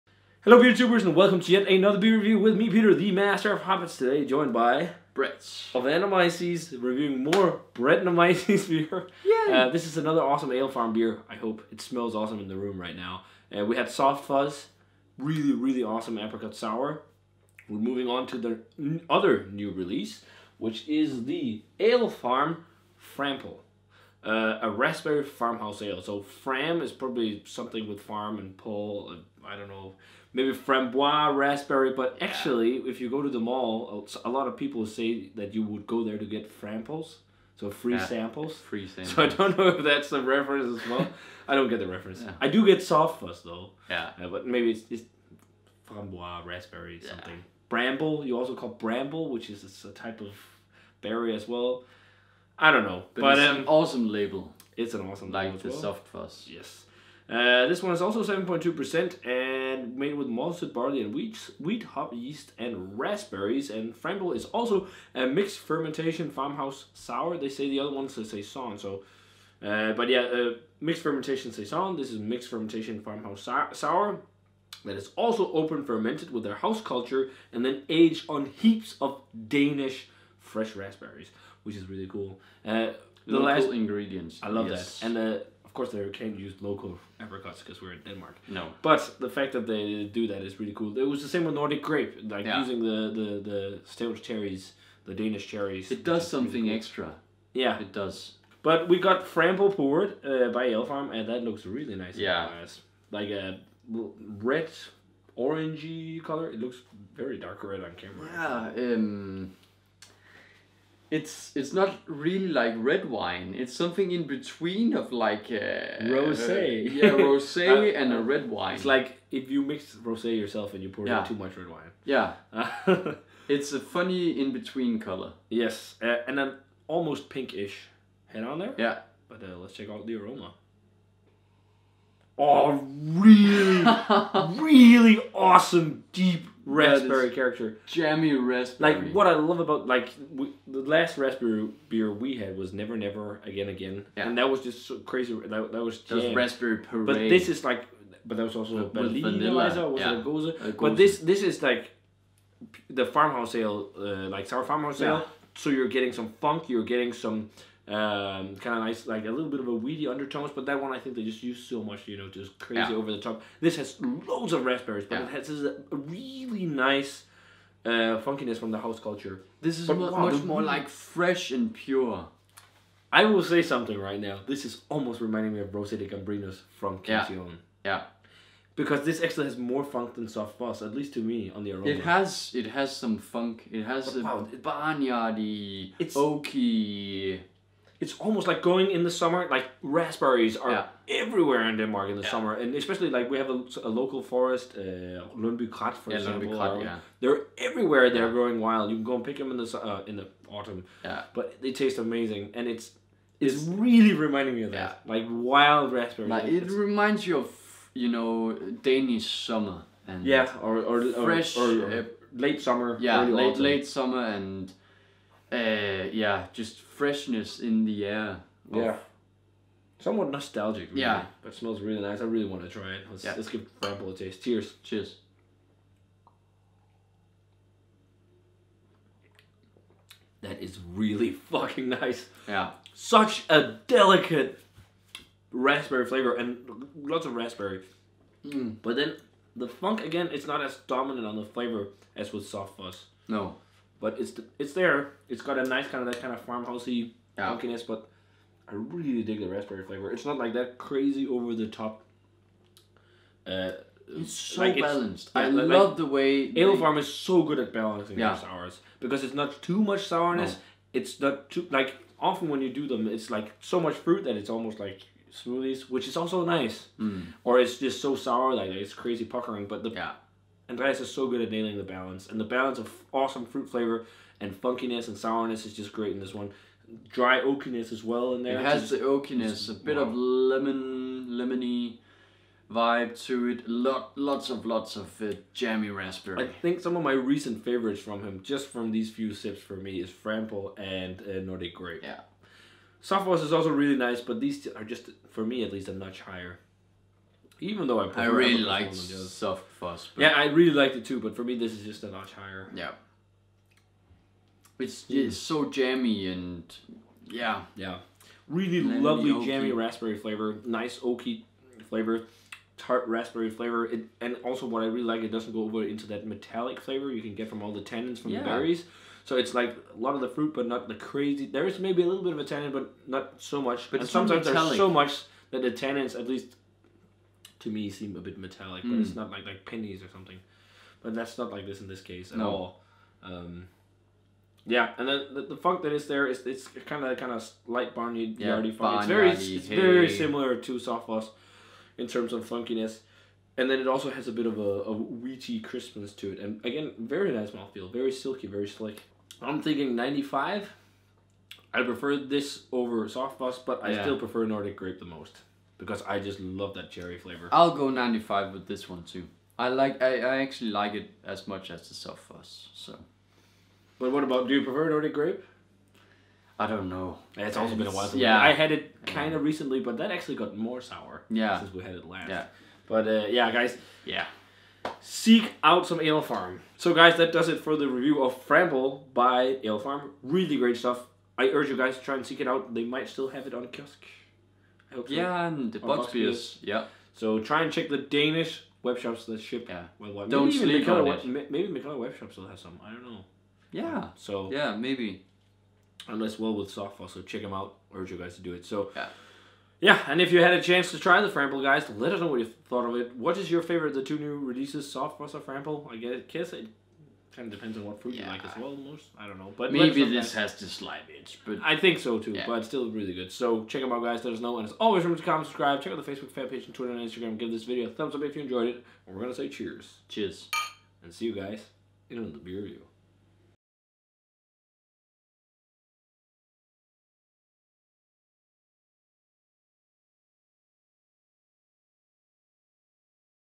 Alefarm Frample | TMOH - Beer Review #2467
Information About This Beer:

Brewery: Alefarm
Beer: Alefarm Frample
Style: Saison
Location: Brewed by Alefarm in Køge, Denmark.
ABV: 7.2%
Rating: 95/100